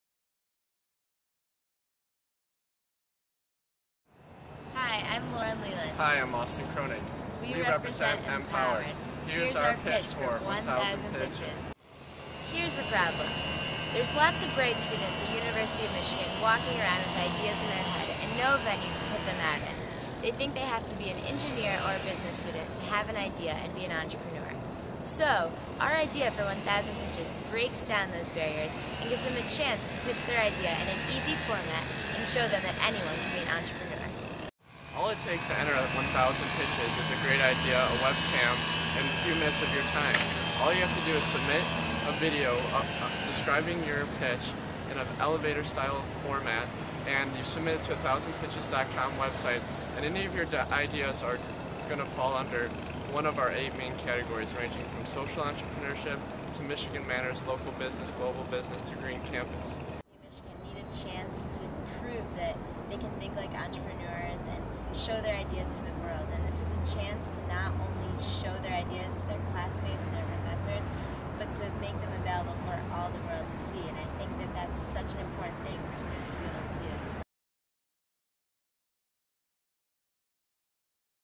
1000 pitches video
The University of Michigan's MPowered Entrepreneurship student club is organizing the second annual "1,000 Pitches" competition in fall 2009. This video, produced by Nathan Bomey of AnnArbor.com's Business Review, features MPowered leaders Lauren Leland and Austin Kronig giving their "pitch" for the event. for more.